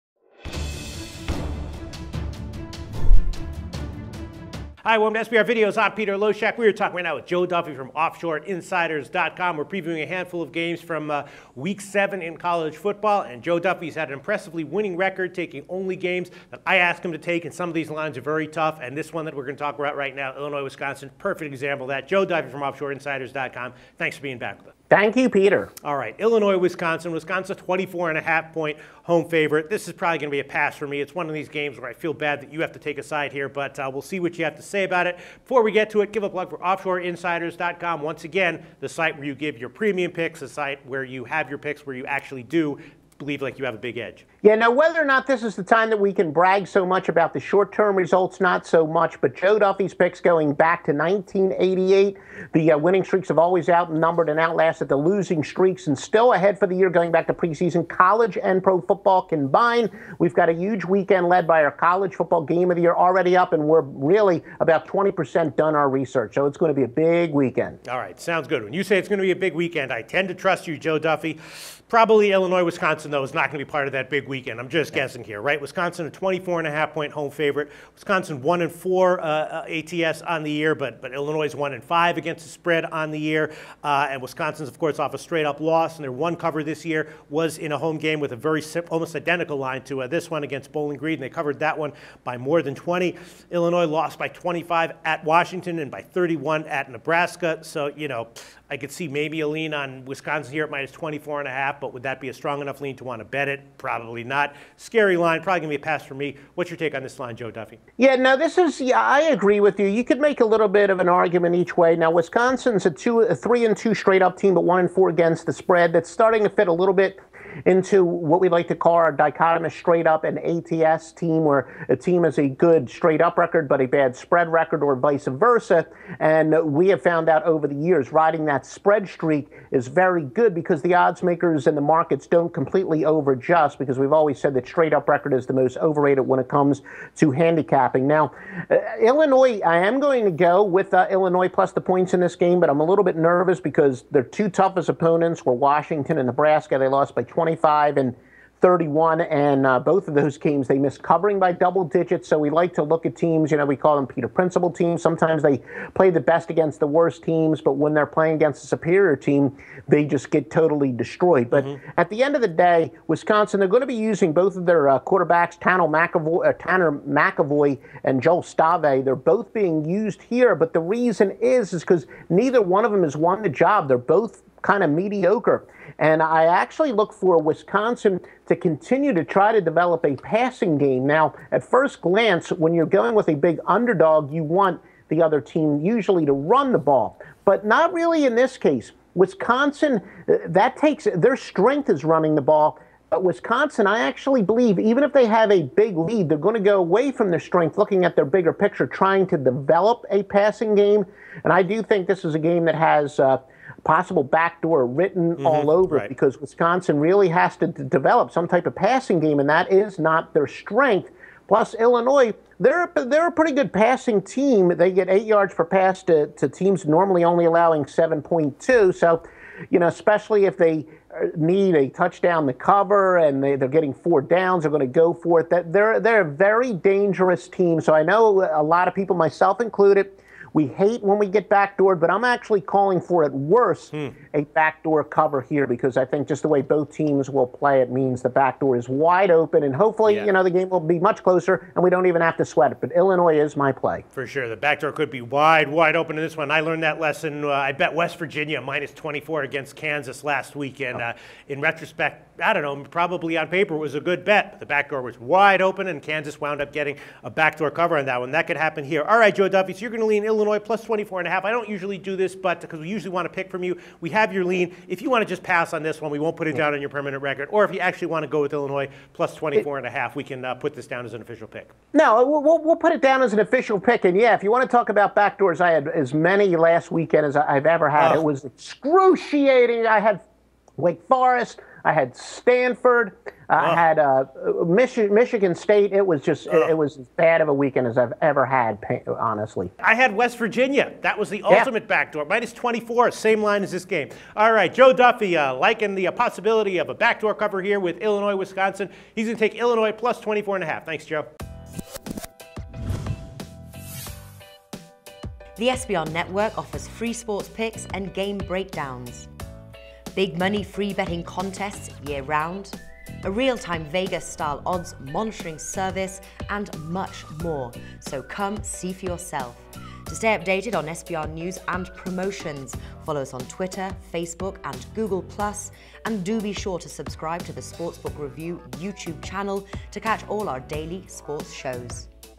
Illinois Fighting Illini vs Wisconsin Badgers Preview: Big Ten Football Picks w/ Joe Duffy, Loshak
The Wisconsin Badgers will be a huge home favorite over the Illinois Fighting Illini in college football Week 7.